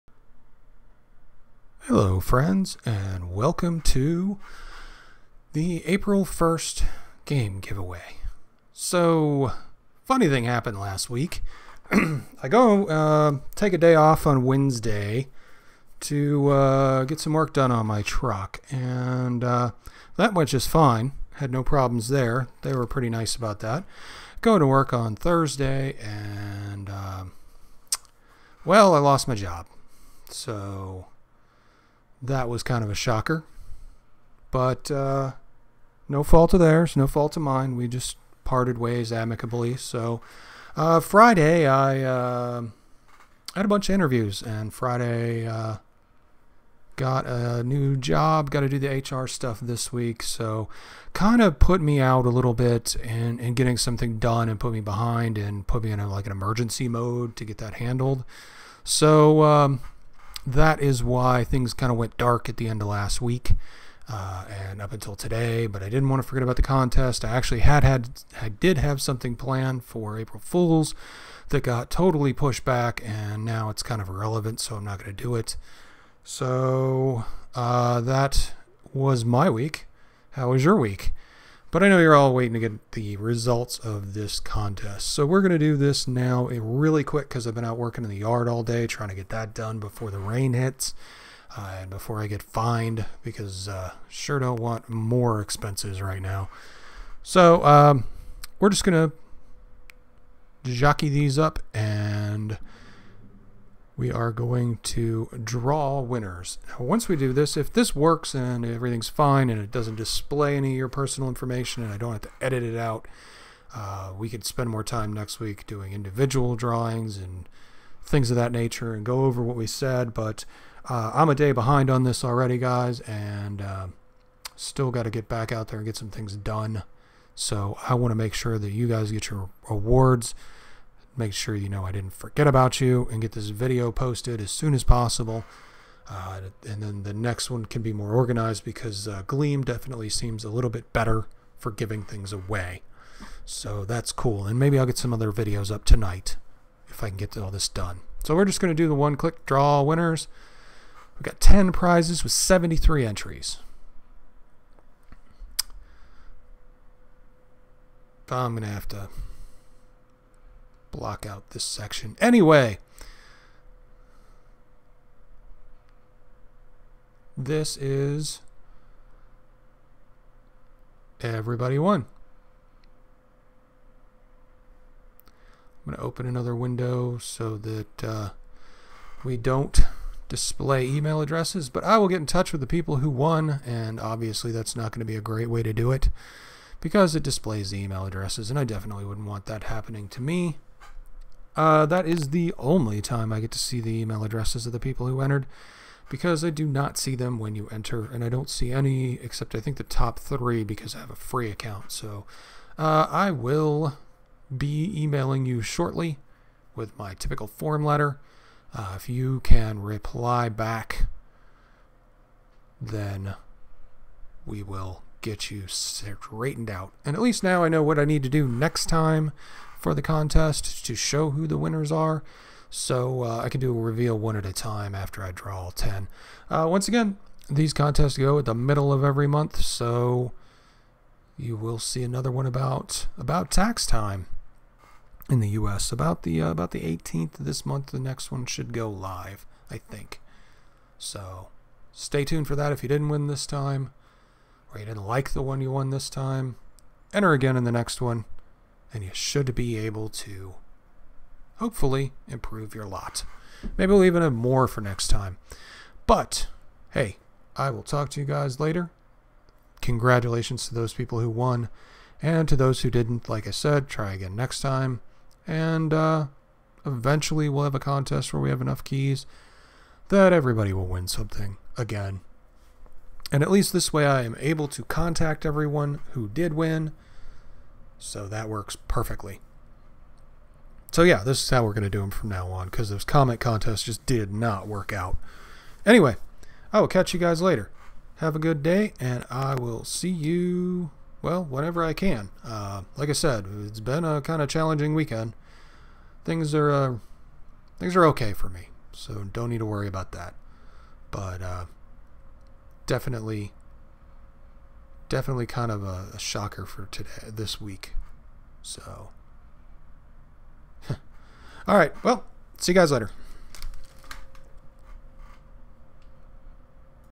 4-1-17 Steam Key contest results
Quick and dirty drawing of the 10 winners , keys have been sent.  the Gleam.io contests seem to work a bit better than the comment contests for sure.  Next contest will be mid-April and should be a bit more organized and with a bit more fanfare.  Personal issues have demolished my week this week but I did want to honor my commitment to give away these keys.